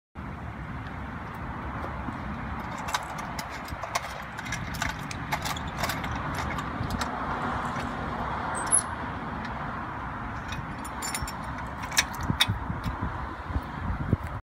PF 06
Installation of our new "All Steel" Wheel Immobilizer.

PF-06S model shown here installed on a small SUV.